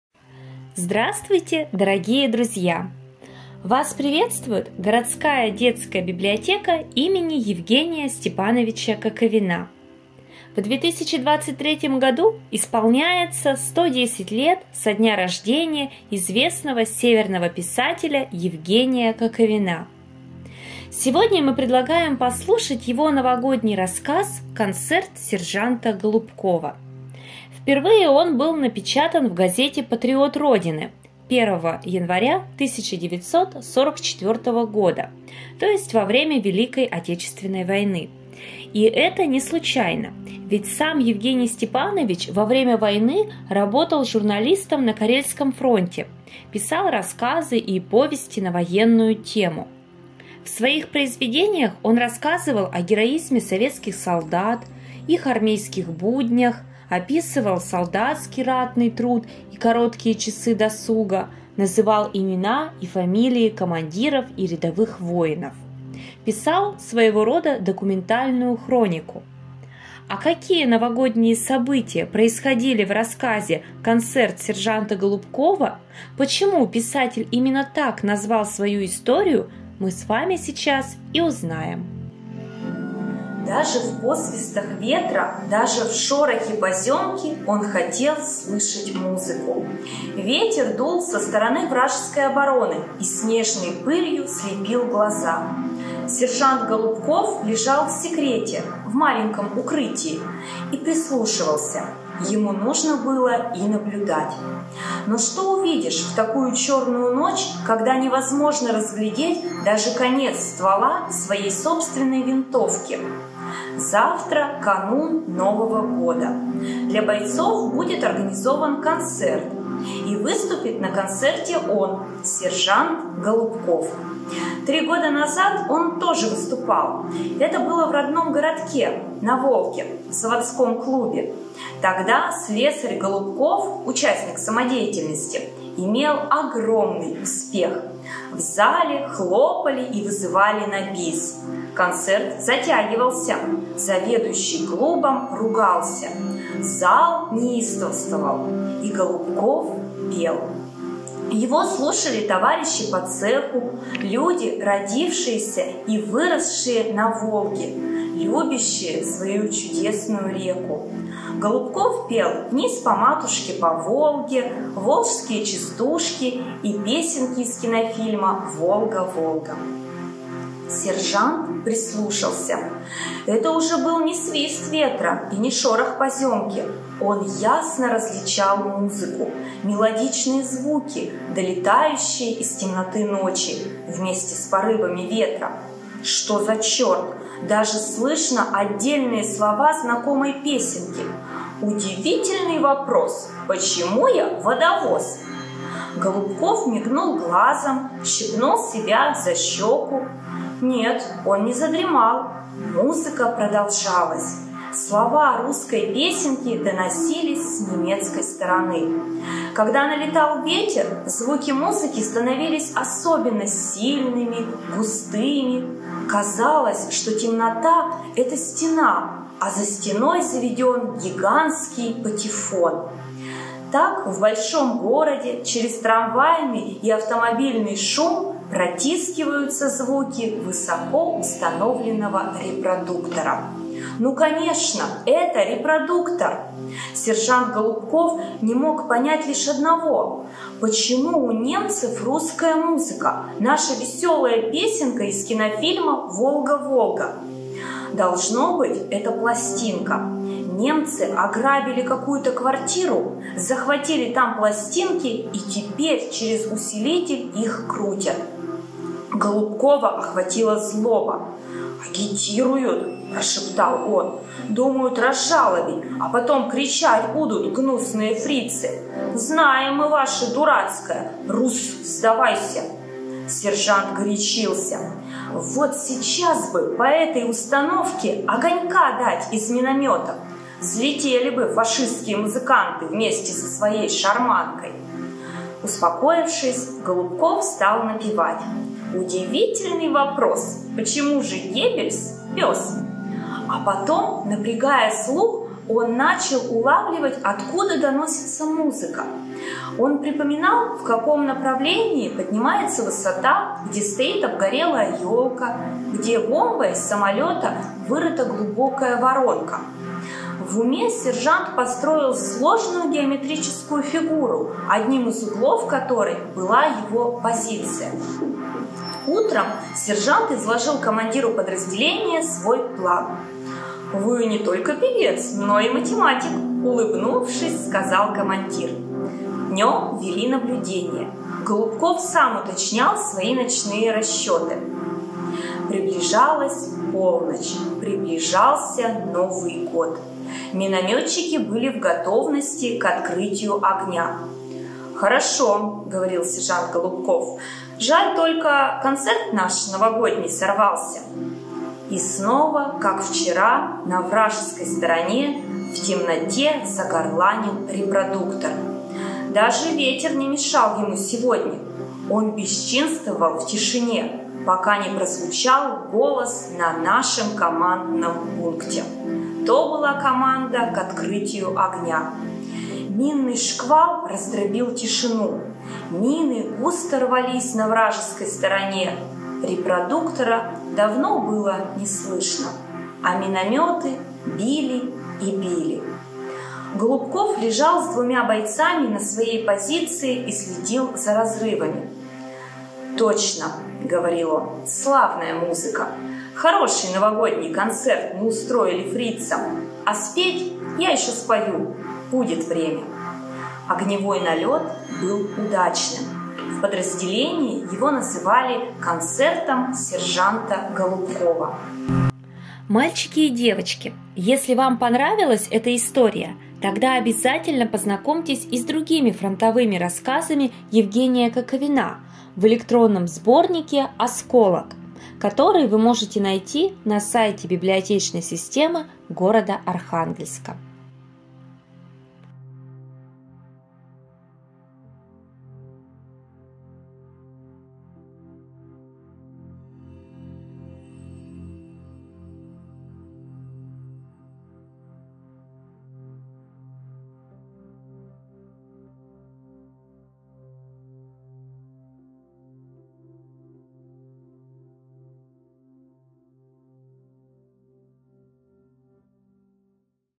Евгений Коковин «Концерт сержанта Голубкова»
К 110-летию со дня рождения известного северного писателя Евгения Коковина Городская детская библиотека № 1 имени Е. С. Коковина предлагает юным читателям послушать его новогодний рассказ «Концерт сержанта Голубкова».